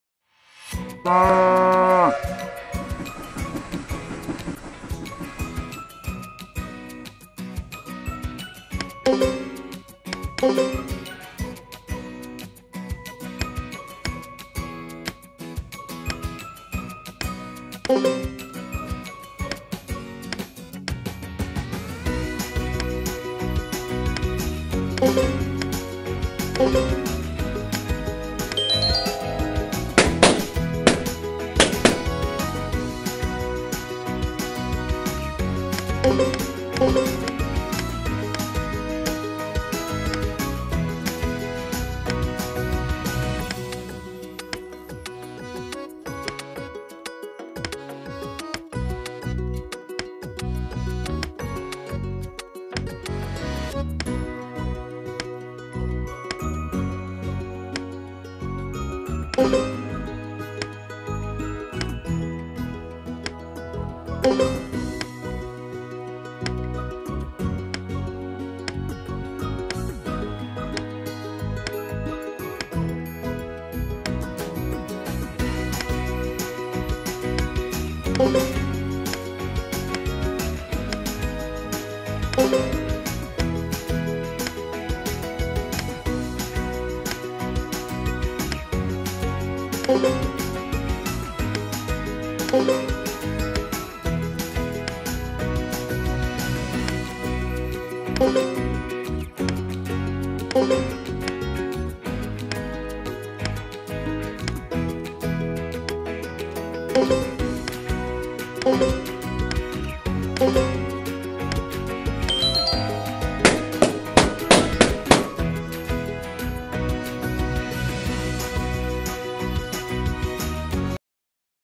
Mémoire jeux d'enfants
Basé sur un jeu traditionnel, Mémoire est amusement éducatif. Pendant la lecture, les enfants apprennent à se concentrer, reconnaître des formes et d'exercer leur mémoire.